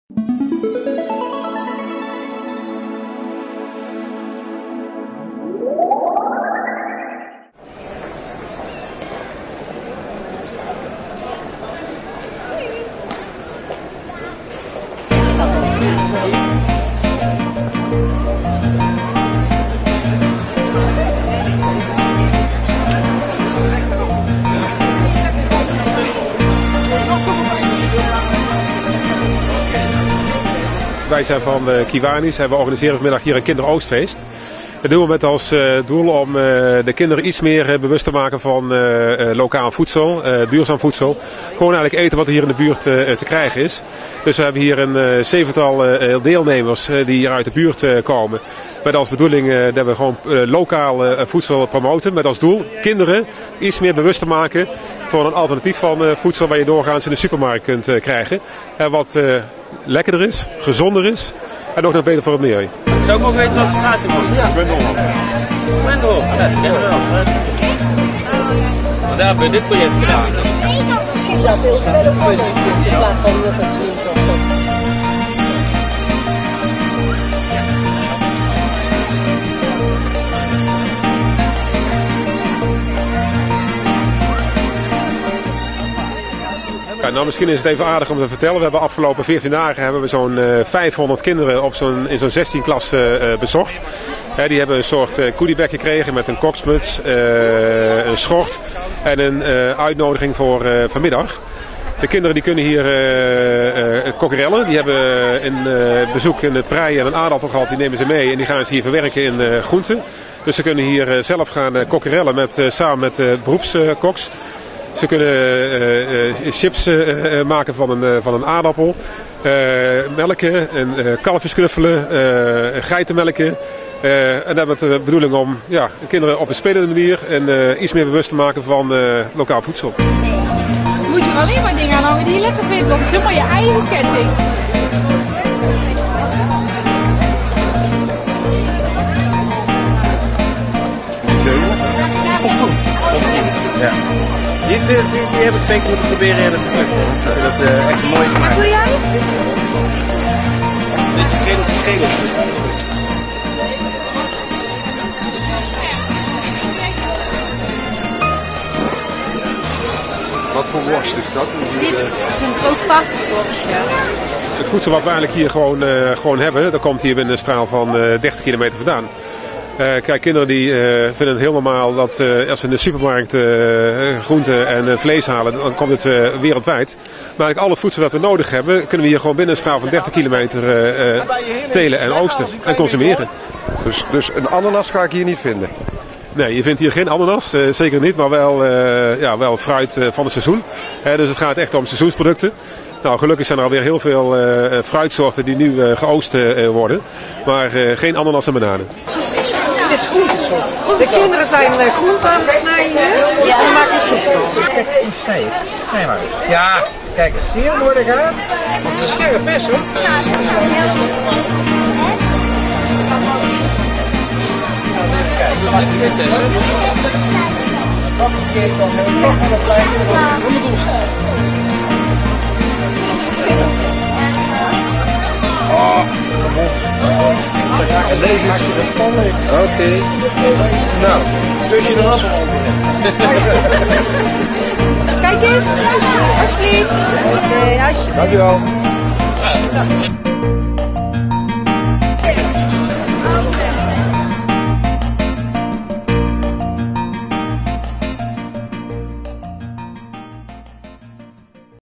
TilburgWebTV - Kinderoogstfeest Pieter Vreedeplein Tilburg 23-09-2012
Kiwanisclub Tilburg organiseerde zondag 23 september op het Pieter Vreedeplein in Tilburg een kinderoogstfeest.